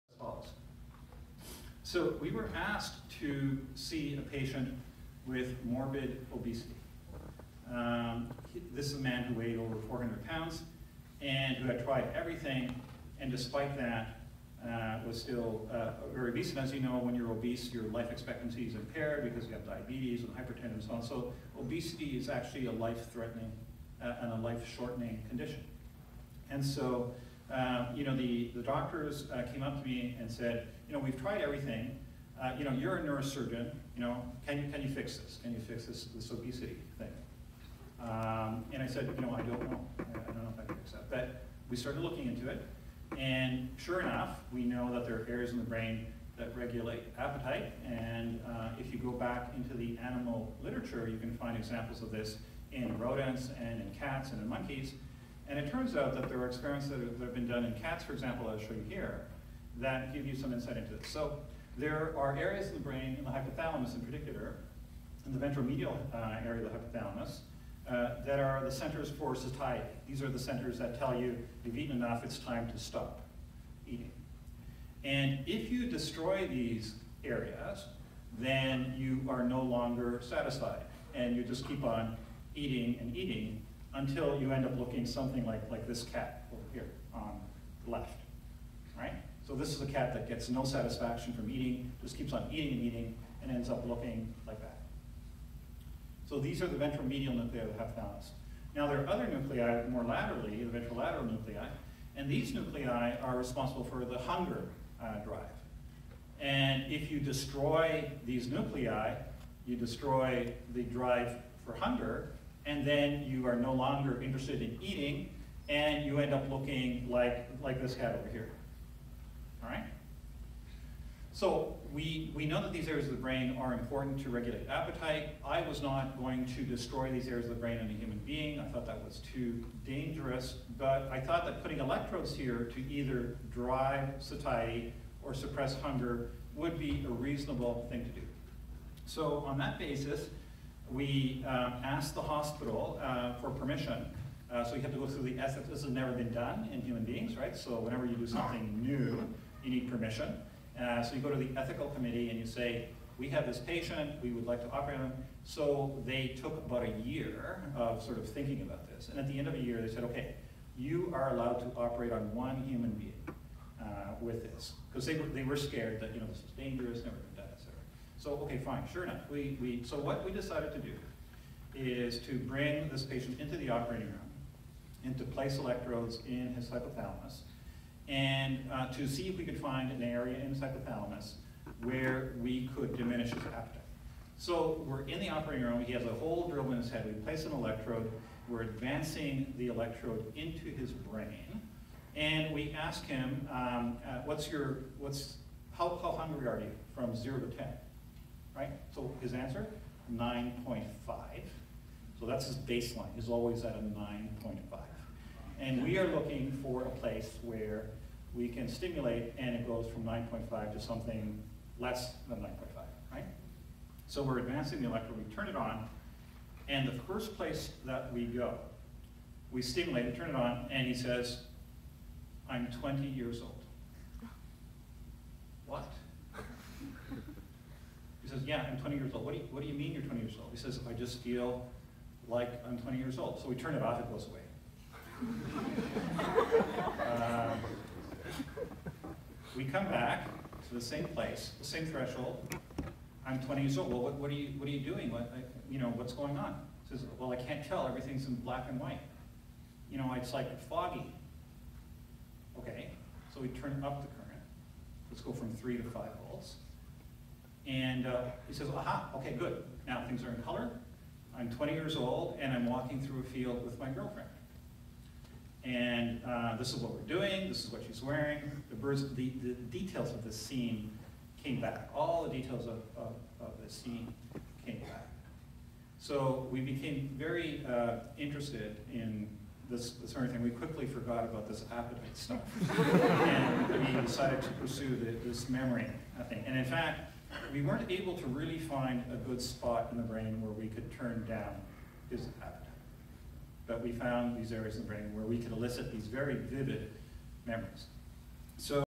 How we discovered how to restore memories
Part of Dr. Andres Lozano's brilliant talk on deep brain stimulation.